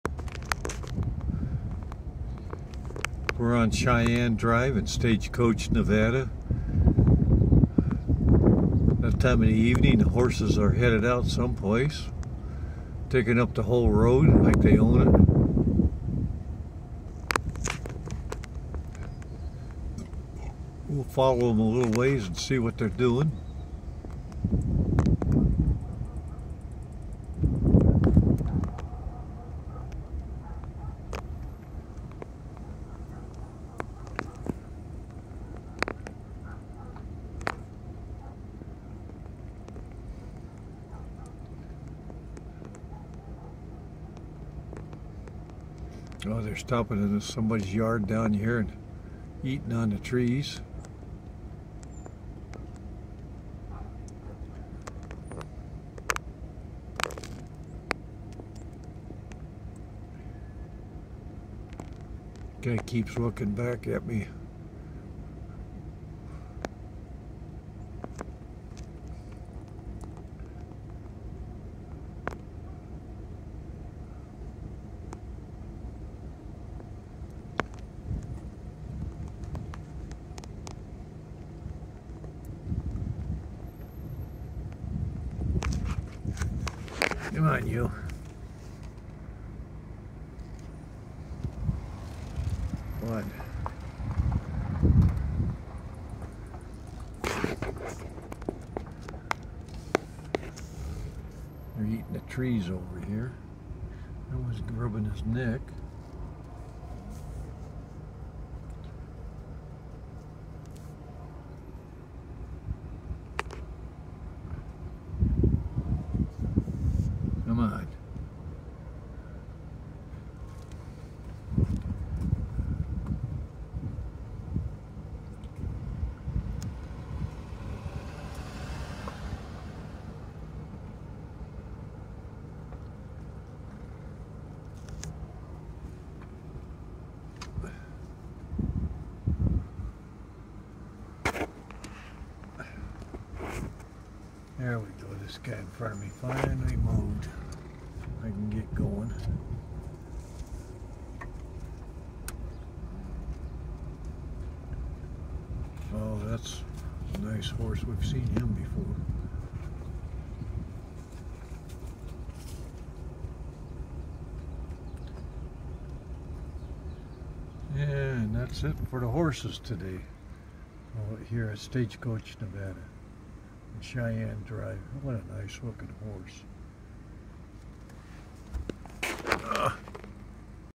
Wild Mustangs Cheyenne Drive Stagecoach Nevada
Mustangs at Stagecoach Nevada on Cheyenne Drive. Up close and personal.Video taken and created 10/7/23.